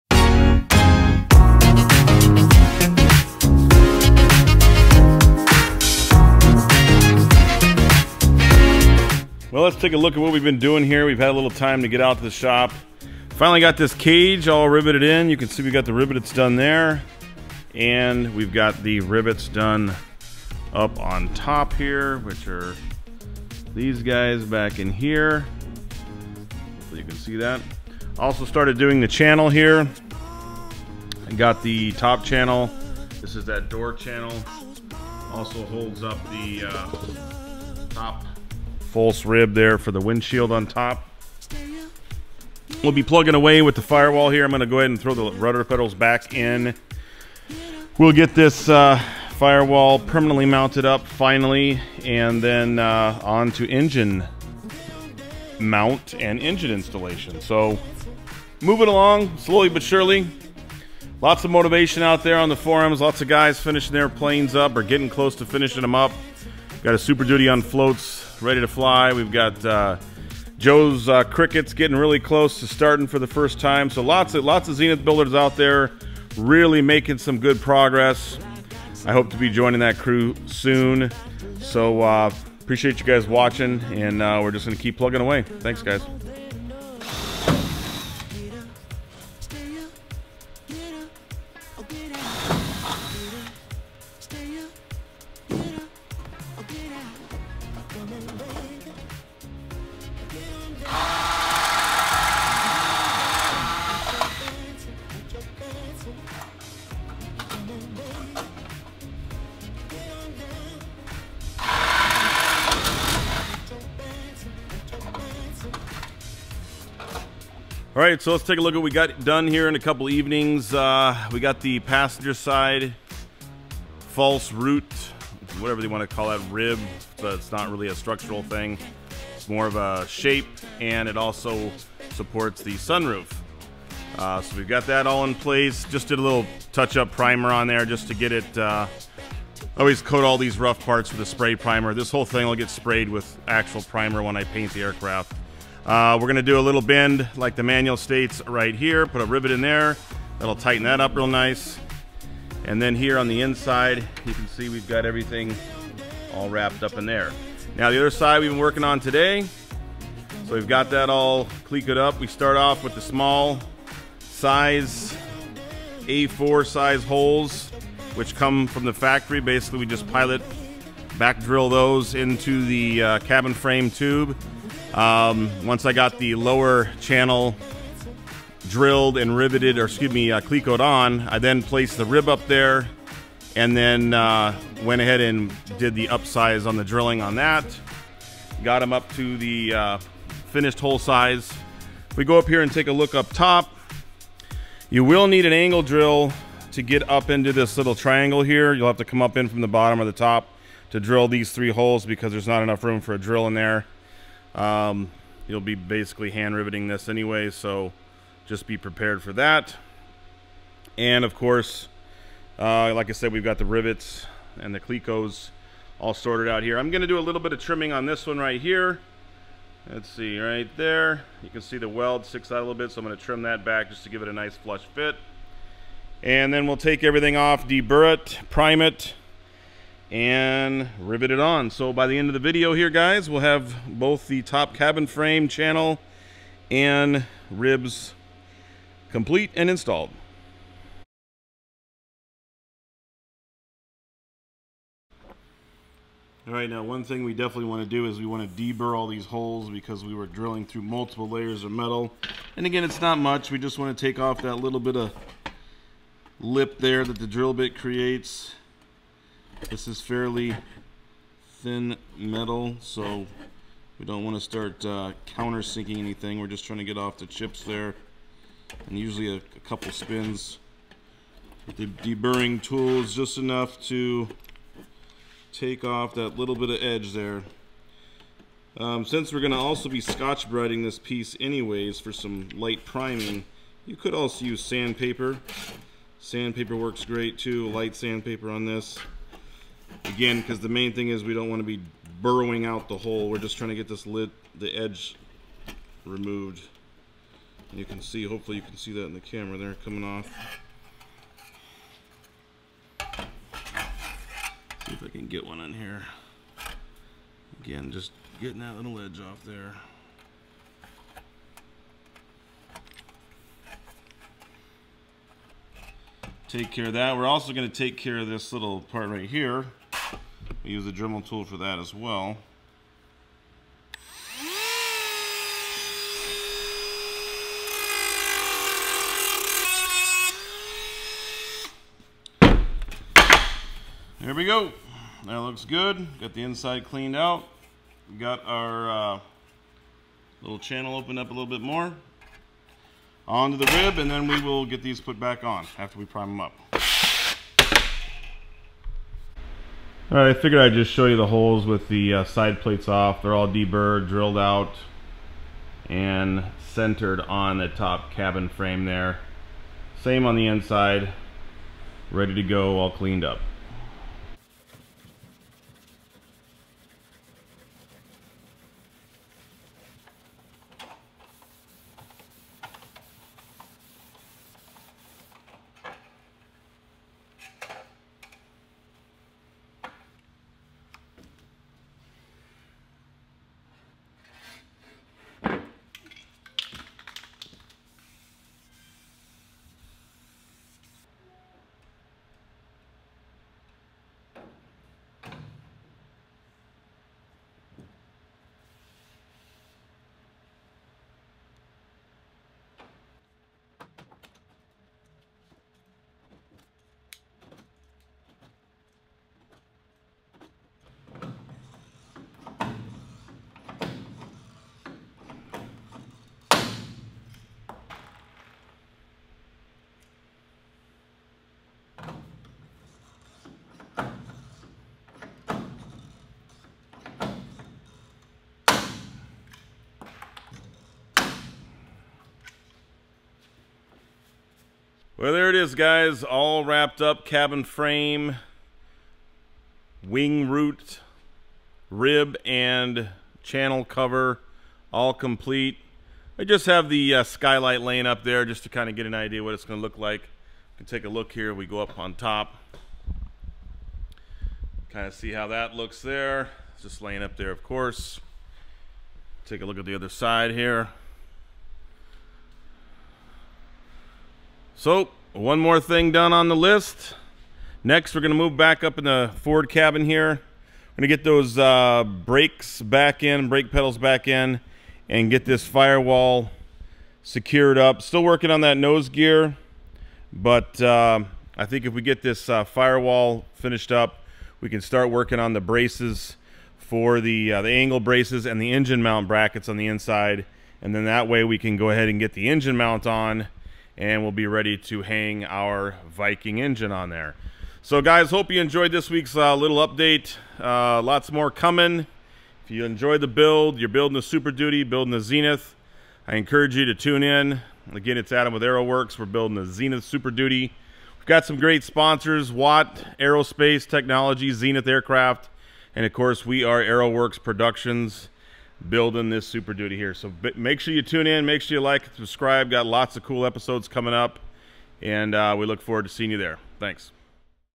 Ep. 37 | Cabin Frame & Wing Root Rib Construction | Zenith Super Duty Aircraft Build
Adam from Aeroworks Productions continues the construction of the  cabin frame and wing root rib in the latest episode of his Zenith Super Duty build series on Youtube.

Vlog Camera
Wireless Mics
Main Camera
Main Lens
Wide Lens